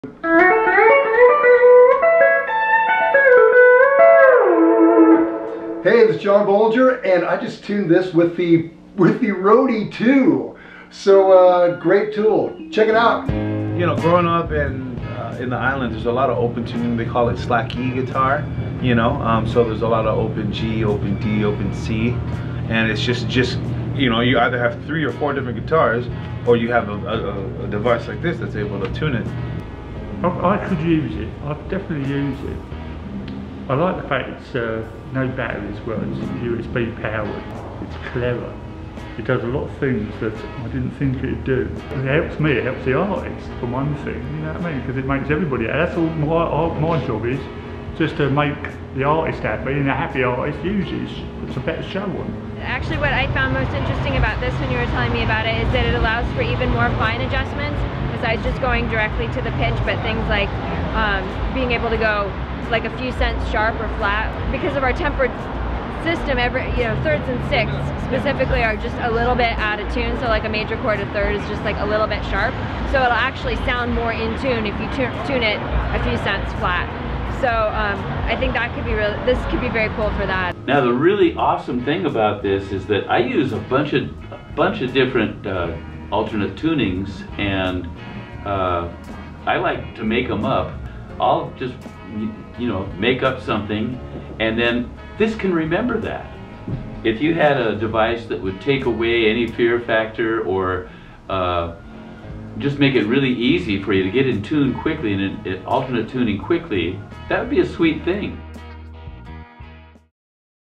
Roadie 2 reviewed by John Bolinger, Mata Grey, Bryan Ferry's tech, Gretchen Menn AND Roger Fisher!!
John Bolinger is a multi instrumentalist famous for recording with 50+ major label artists, Mata Grey is the guitarist in Common Kings, Slouch is Bryan Ferry's tech and has worked on some of the biggest stages in the world, Gretchen Menn is a founding member of Zepparella and Roger Fisher is the founding member of Heart.  A lot of talent is in this video and one thing they have in common besides a love of music is a love for Roadie 2.

Check out the update this video is a part of at the Roadie 2 Kickstarter campaign